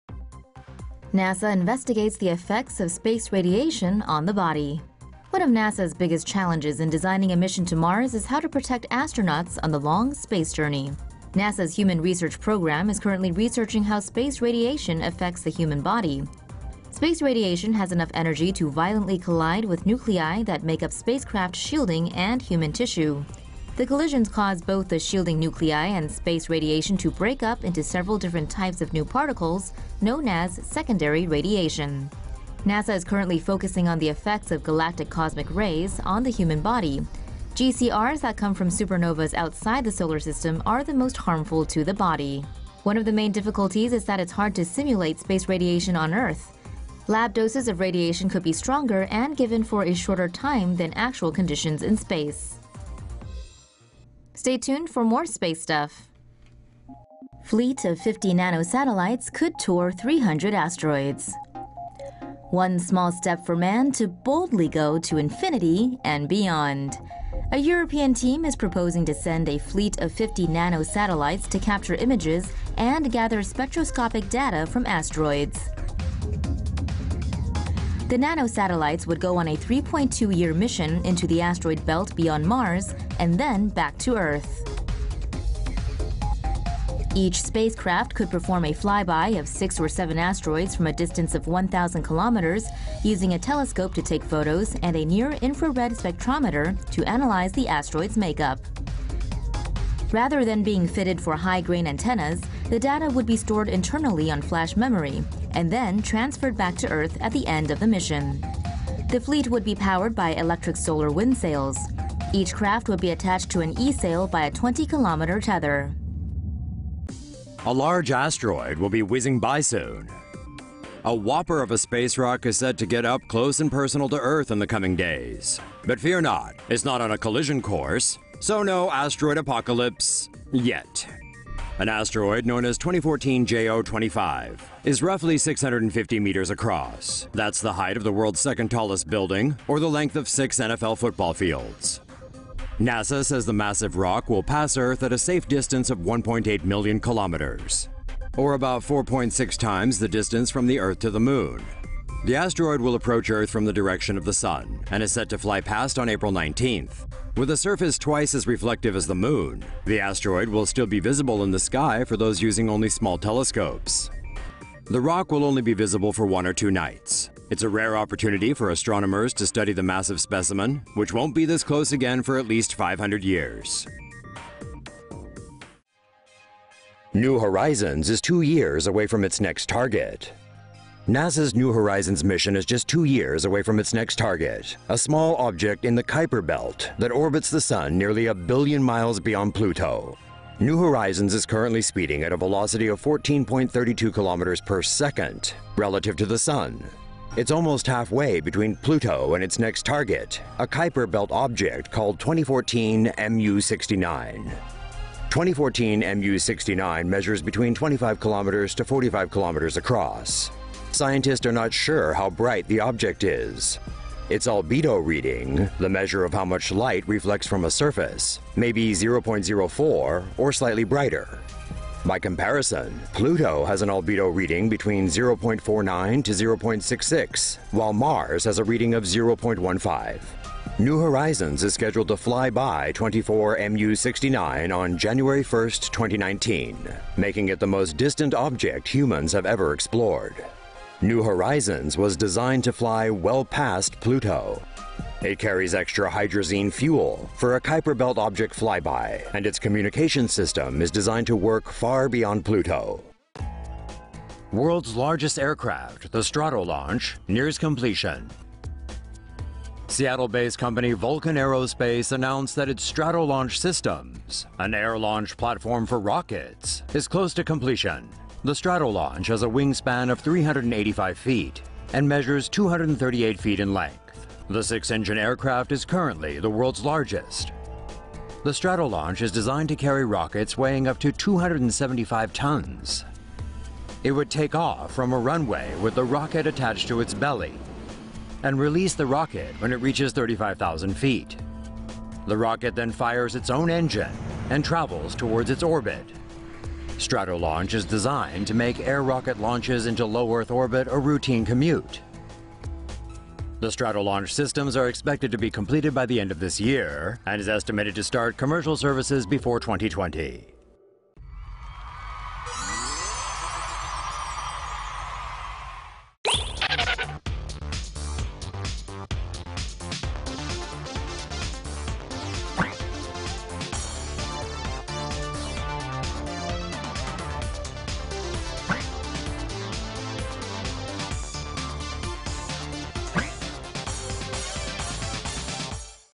Mars missions: NASA studying effects of space radiation on the human body - TomoNews
WASHINGTON — One of NASA’s biggest challenges in designing a mission to Mars is how to protect astronauts on the long journey through space.

NASA’s Human Research Program is researching how space radiation affects the human body.

“One of our biggest challenges on a mission to Mars is protecting astronauts from radiation,” said NASA Space Radiation Element Scientist Lisa Simonsen, reported Science Daily. “You can’t see it; you can’t feel it. You don’t know you’re getting bombarded by radiation.”

Space radiation has enough energy to violently collide with nuclei that make up spacecraft shielding and human tissue, according to Science Daily.

The collisions cause both the shielding nuclei and space radiation to break-up into several different types of new particles known as secondary radiation.

NASA is currently investigating the effects of galactic cosmic rays (GCRs) on the human body.

One of the main difficulties is that it’s hard to simulate space radiation on Earth. Lab doses of radiation could be stronger and given for a shorter time than actual conditions in space.